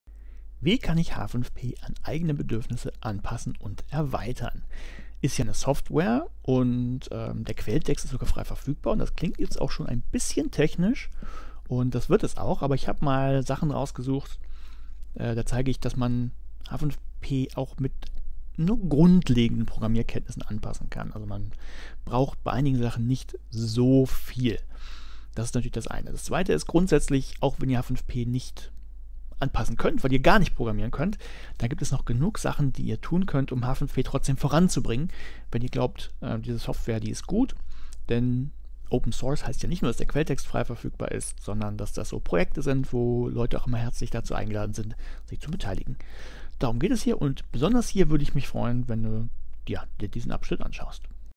OERcamp SummOERschool 2020, H5P: Wie kann ich H5P anpassen?
Dieses Video ist Bestandteil des kostenlosen und offen lizenzierten Kurses zu H5P Schau gerne dort vorbei, denn dort gibt es den hier fehlenden Kontext und auch Zusatzmaterial.